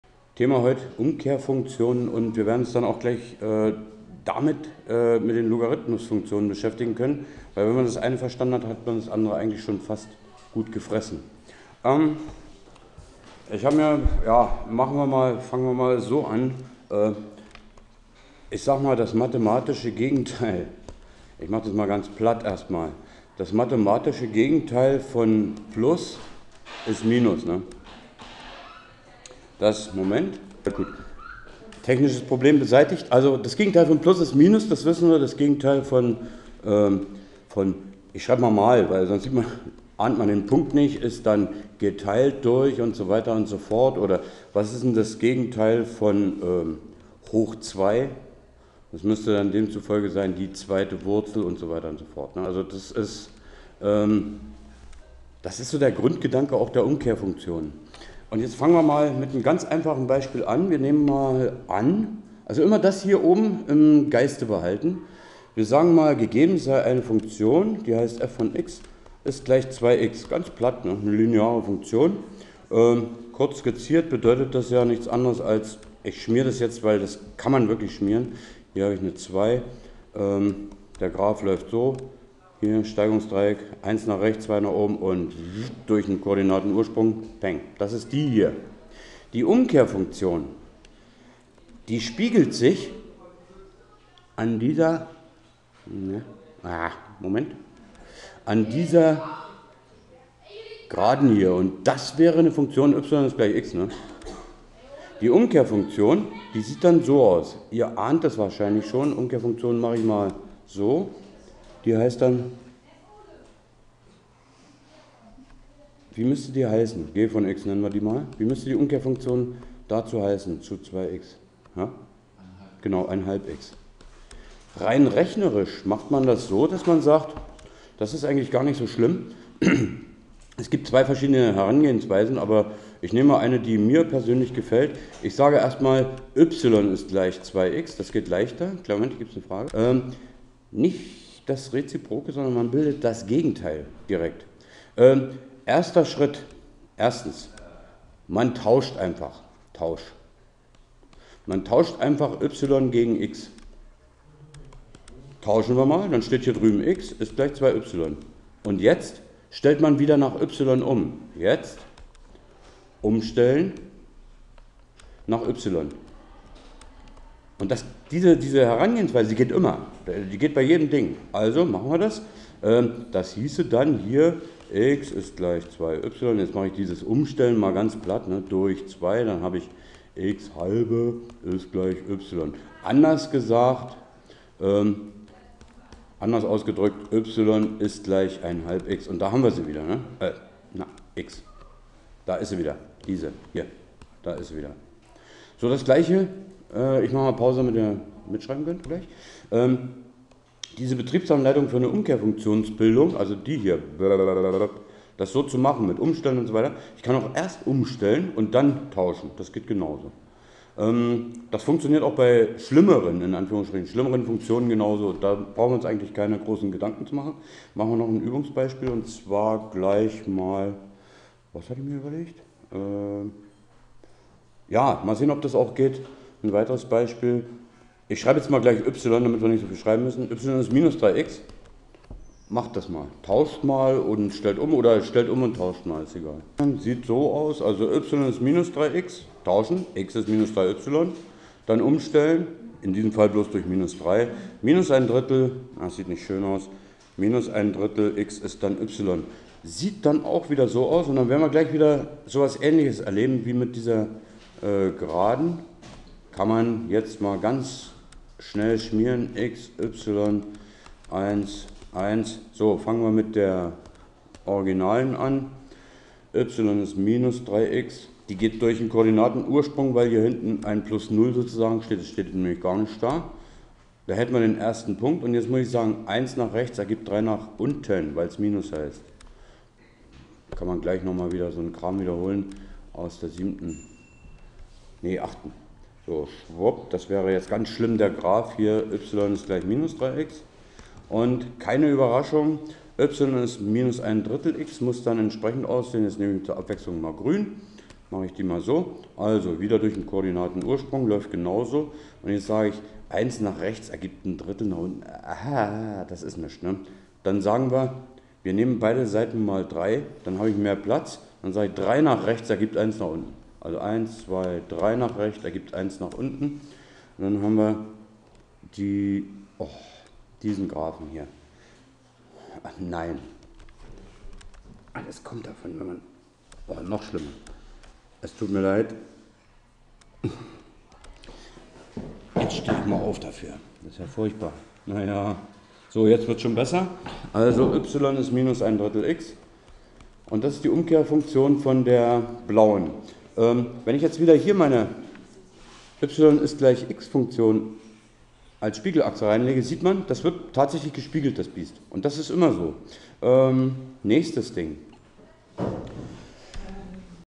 Umkehrfunktionen einfach bilden, Teil 1, zuerst das Prinzip mit linearen Funktionen
Bitte, helfen Sie uns Endlich wieder (Sapere aude!) Umkehr-Funktionen bilden ist einfach, weil man nur X und Y vertauscht und dann wieder nach Y umstellt / auflöst. Zur Erinnerung: Exponentialfunktionen heißen so, weil das "X" im Exponenten steht. Bei Potenzfunktionen ist das "X" die Basis.

11 2014_05_23 :zzzzz_intern